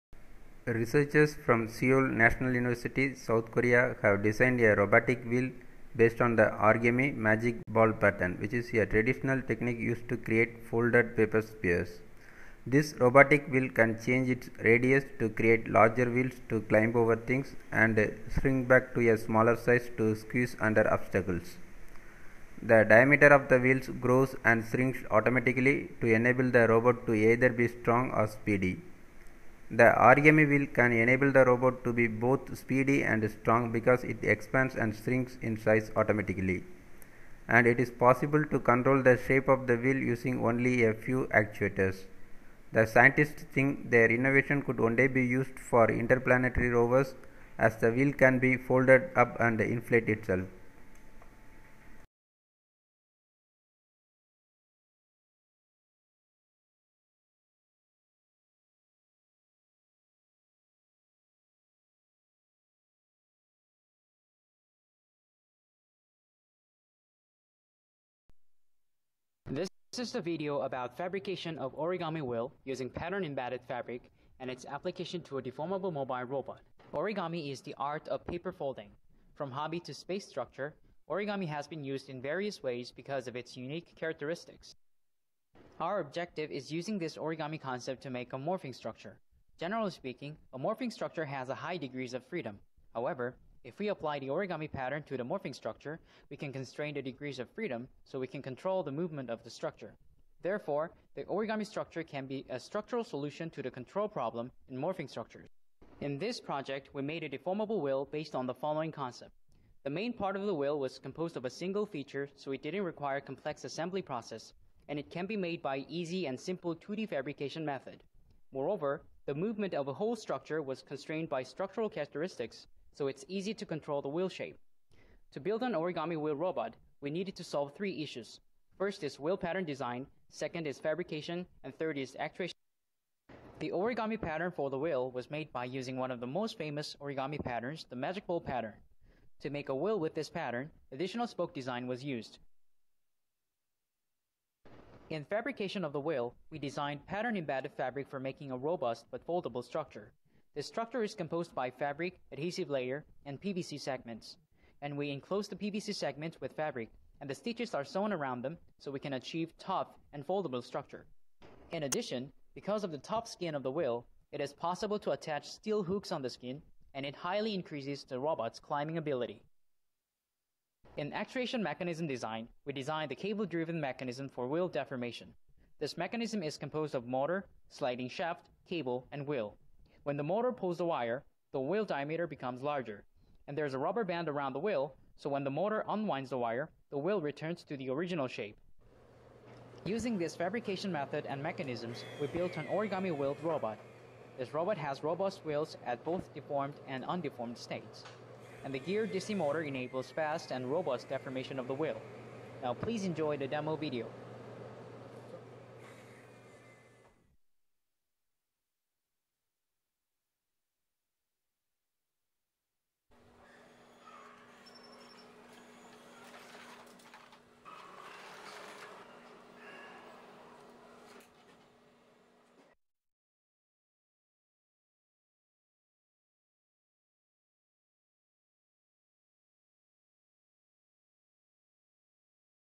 Deformable Mobile Robot with Origami Wheels which can expand and shrink in size automatically
Researchers from Seoul National University, South Korea, have designed a robotic wheel based on the origami  'magic ball pattern,' which is a traditional technique used to create folded paper spheres.

This Robotic wheel can change its radius to create larger wheels to climb over things, and shrink back to a smaller size to squeeze under obstacles

The diameter of the wheels  grows  and shrinks  automatically to enable the robot to either be strong or speedy.

The origami wheel can enable the robot  to be both speedy and strong because it expands and shrinks in size automatically.

And, it is possible to control the shape of the wheel using only a few actuators.

The scientists think their innovation could one day be used for interplanetary rovers as the wheel can be folded up and 'inflate' itself.